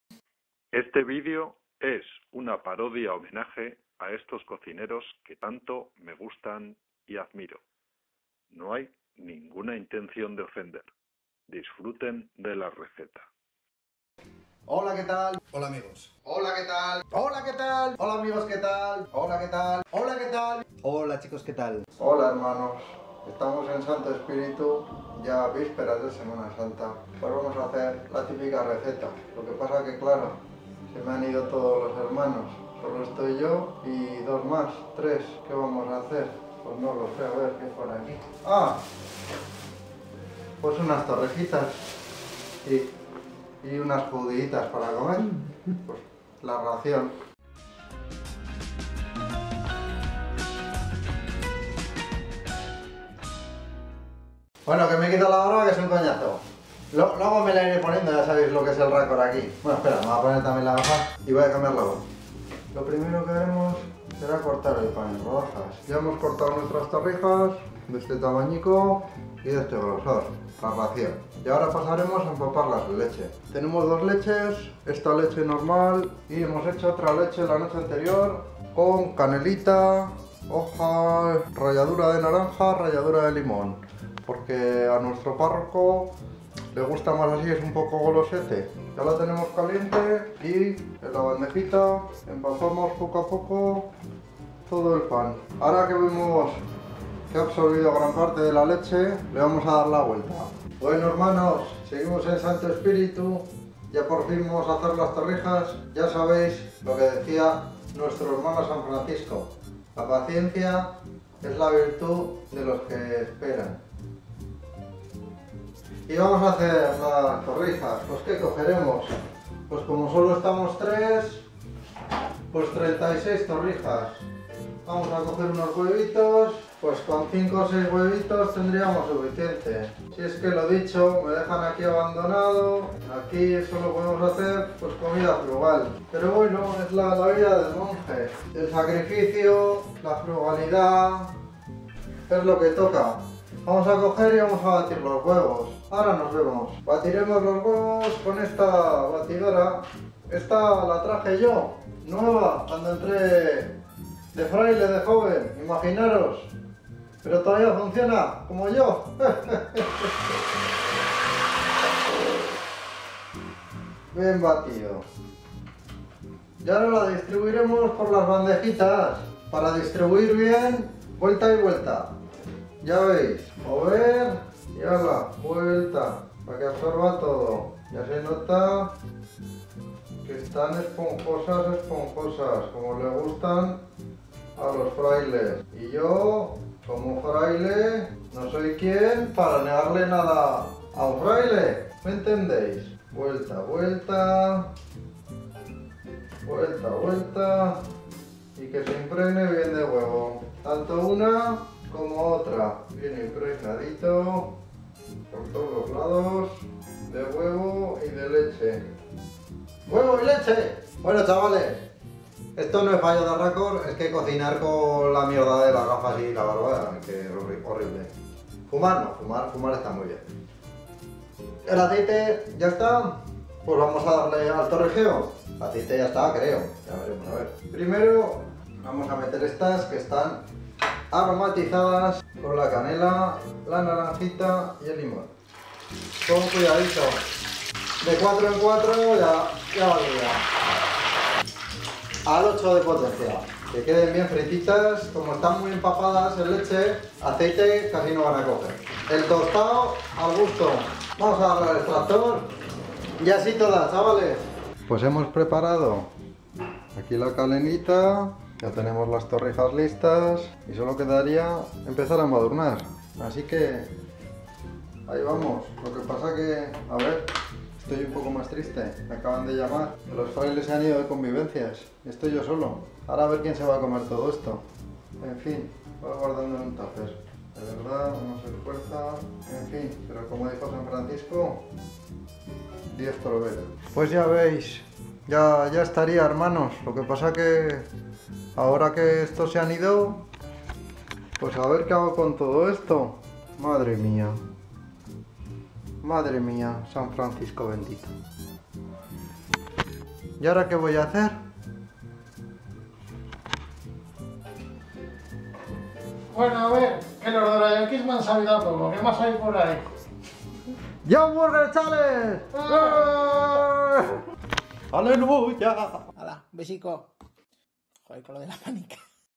Cocinando con invitados hoy Fray Angel
Hoy viene a cocinar con nosotros el más famoso cocinero Franciscano, que nos deleitaré con unas fabulosas torrijas.